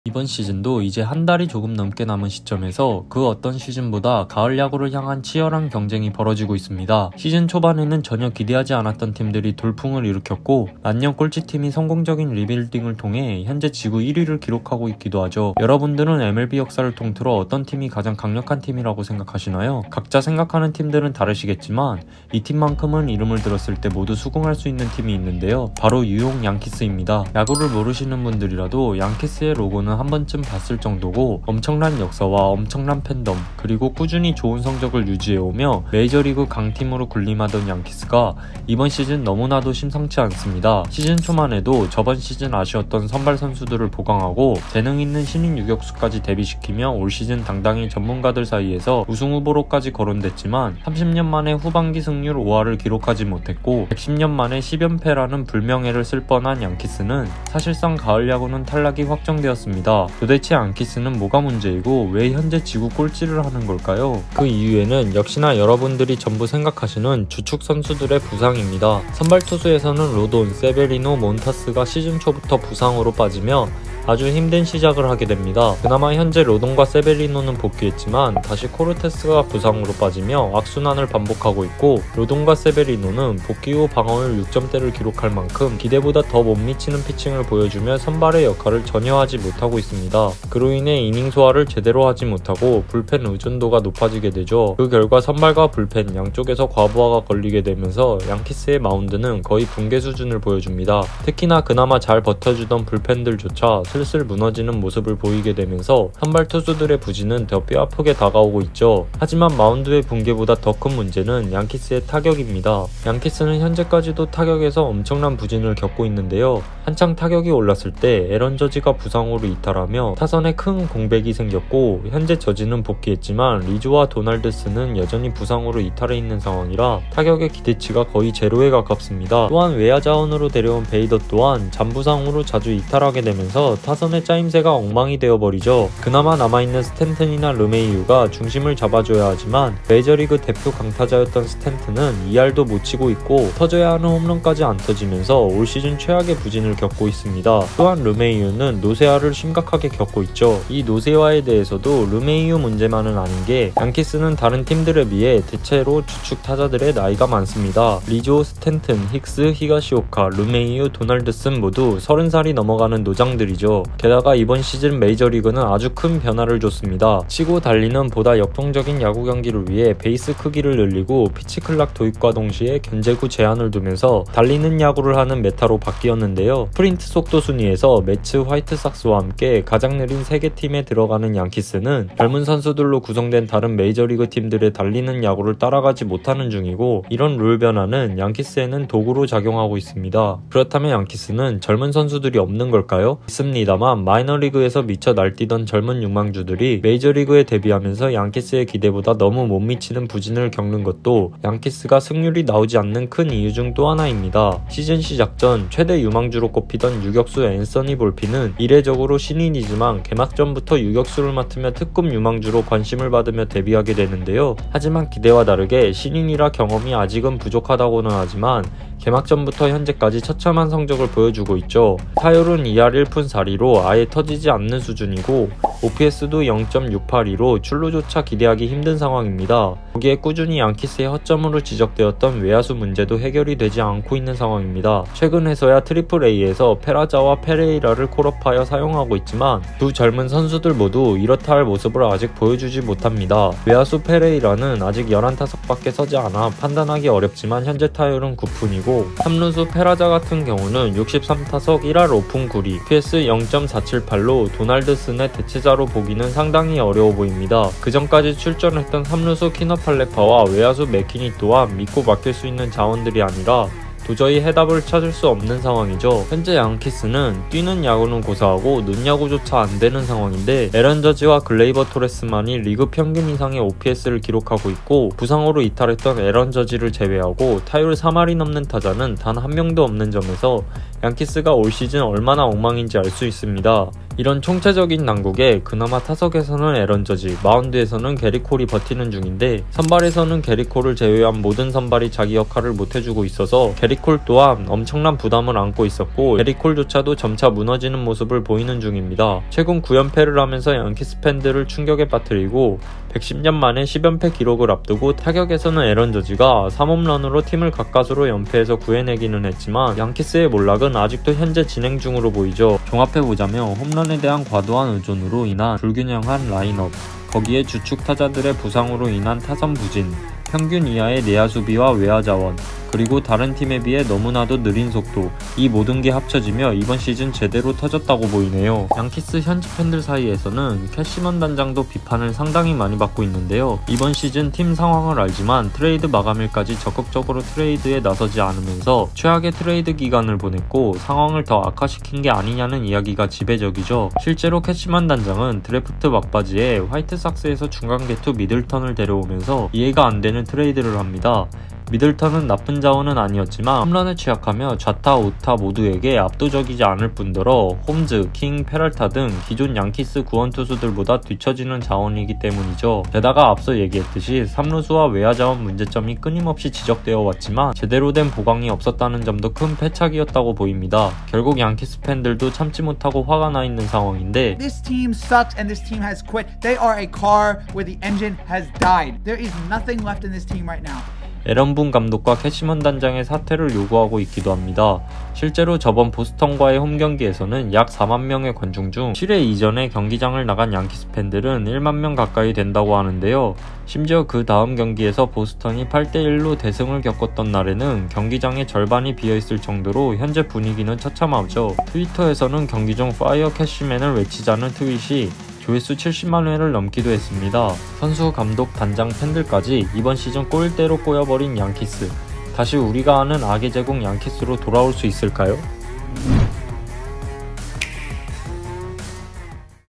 MLB 최악의 팀이 되어버린 양키스, 어쩌다가 꼴찌를 하고 있을까?
시즌 전만 해도 강타자들이 즐비한 라인업과 더불어 선발 선수들까지 보강하며 올 시즌 유력한 우승 후보로 떠올랐던 양키스는 현재 어쩌다가 승률 5할도 넘지 못하며 지구 꼴찌를 하게 된걸까요?